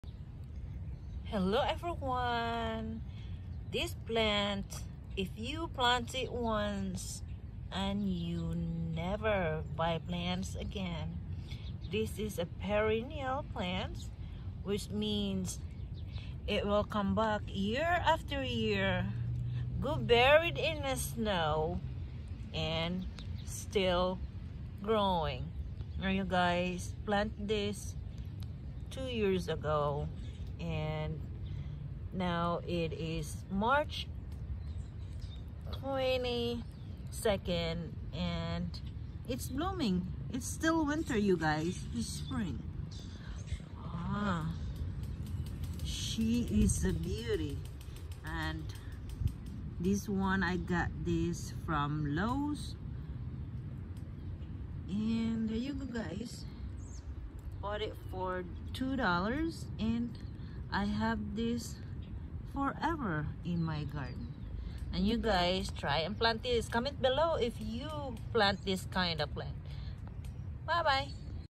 Never Buy Daffodils 🌼 Again
Amazing plant will come back year after year .